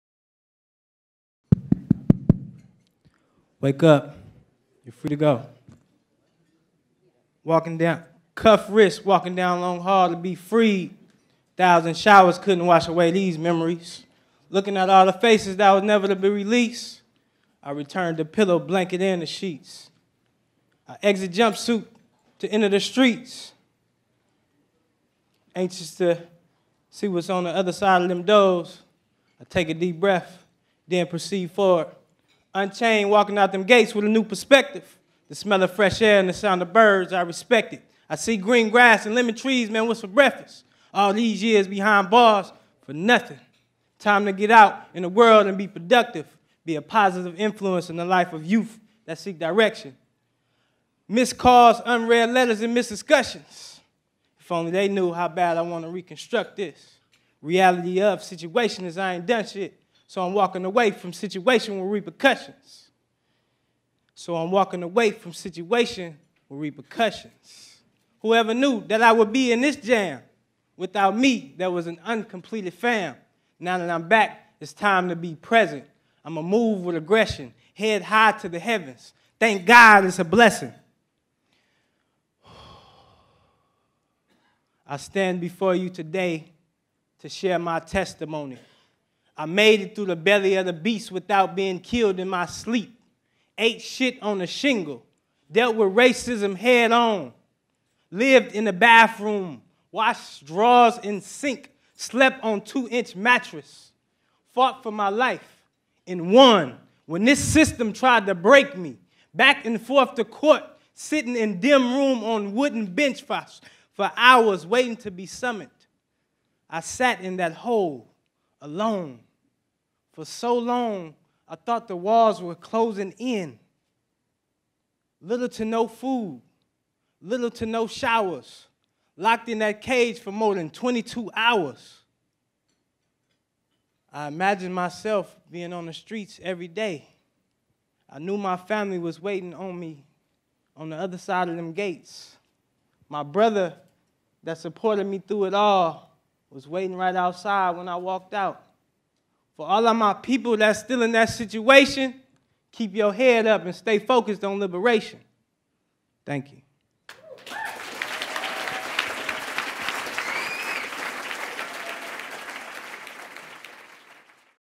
NCIP 2018 SpokenWord by Tj
Spoken word artist Tj Sykes performs at Northern California's Innocence Project's Justice For All Awards Ceremony on 4/12/18 in San Francisco at the Julia Morgan Ballroom.